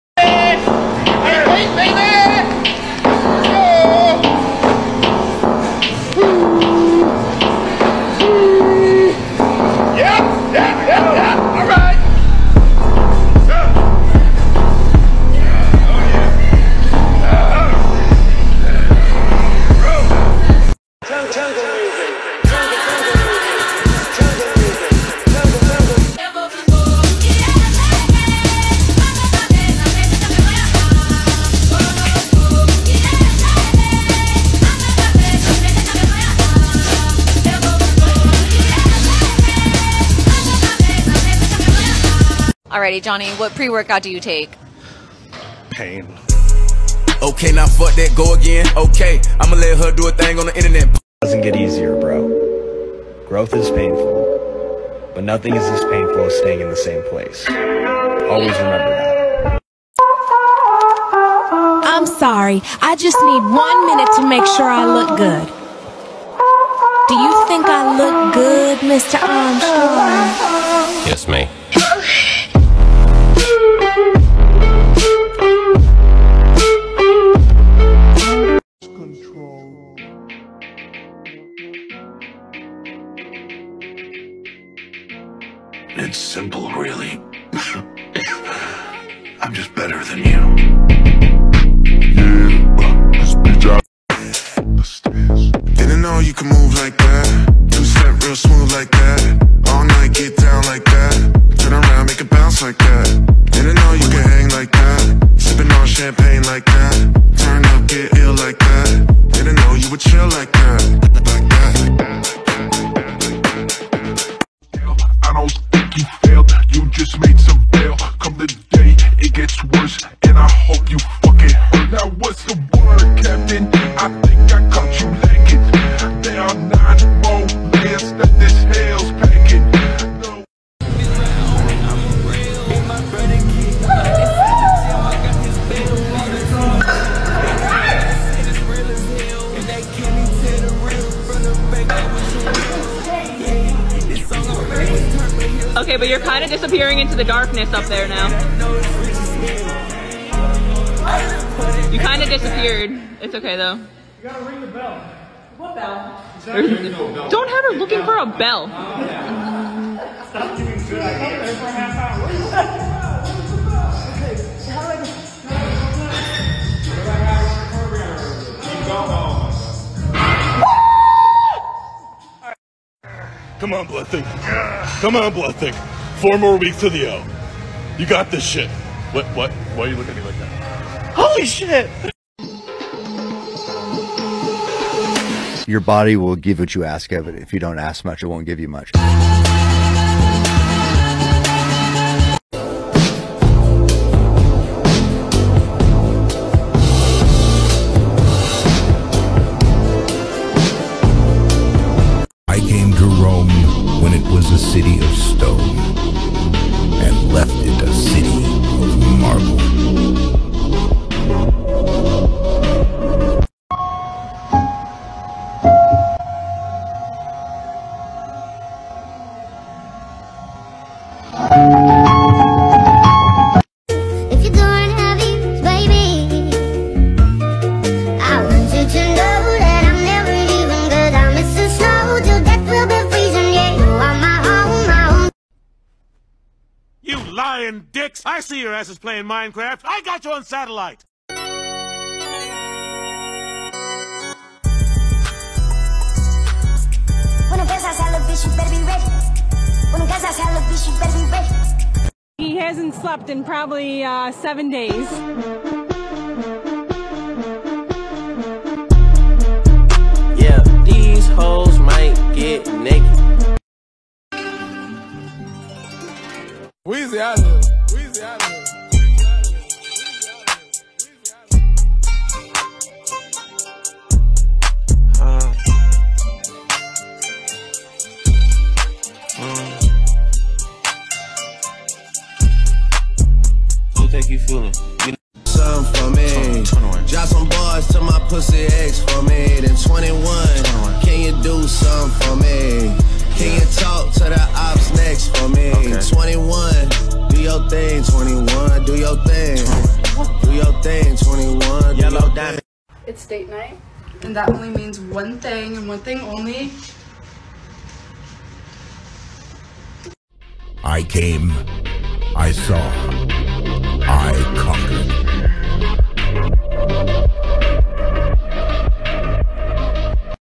5 Minutes of Ripped Guys and Gals. Relatable Tiktoks/Gymtok Compilation/Motivation #73
For all my fellow gym rats.